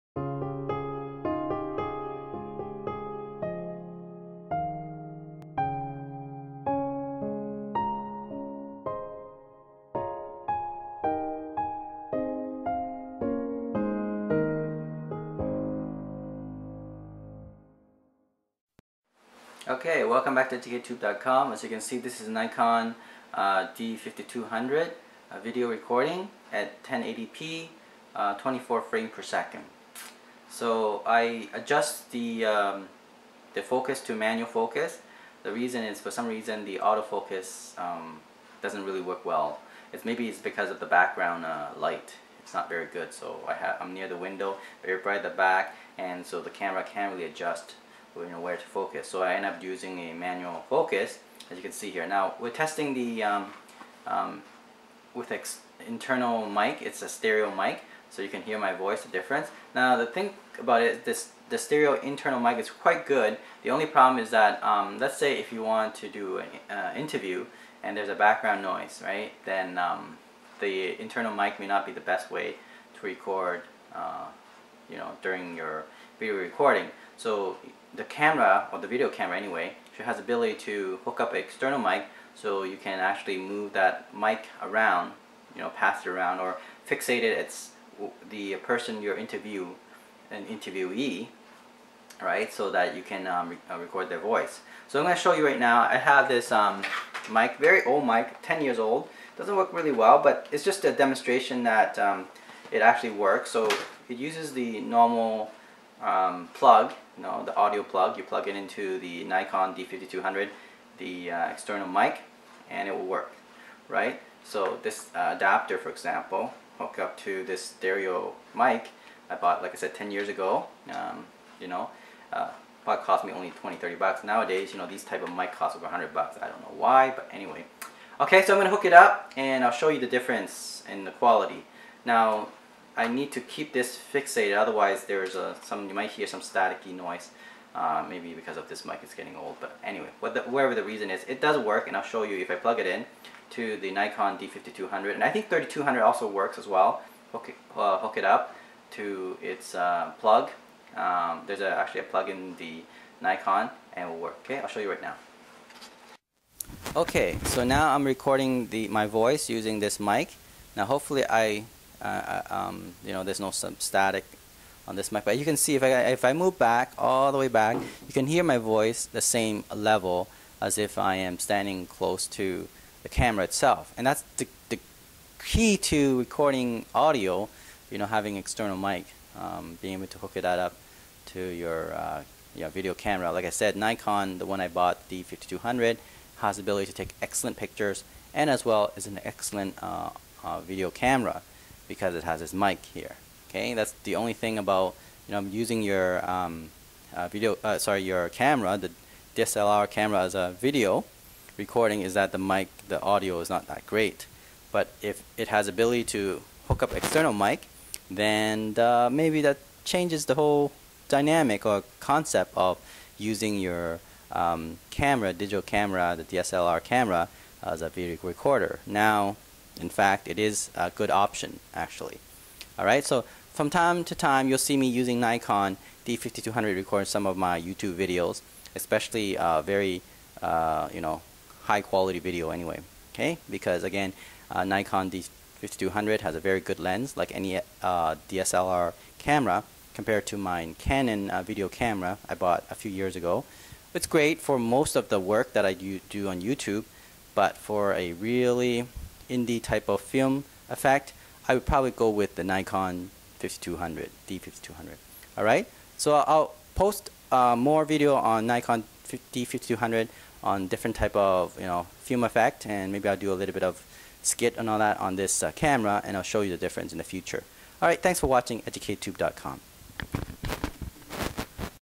Nikon D5200 DSLR Camera - Internal Stereo Mic Vs. External Stereo Mic Comparison Test
Filmed with Nikon D5200 camera:  I am testing the Nikon D5200 DSLR camera internal mic verses the external mic to see if it makes a difference.  The conclusion is that it does and will be very useful for short films or indie films.  Cool! Sipski :)